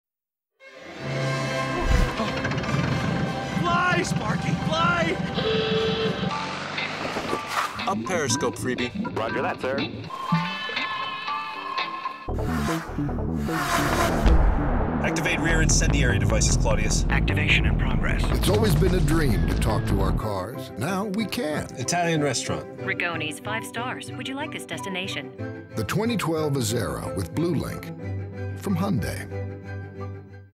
Azera Talk to My Car
Azera Talk to My Car For Hyundai Created by Innocean Worldwide Americas Silver ADDY  Award Television   Regional  National   Single Spot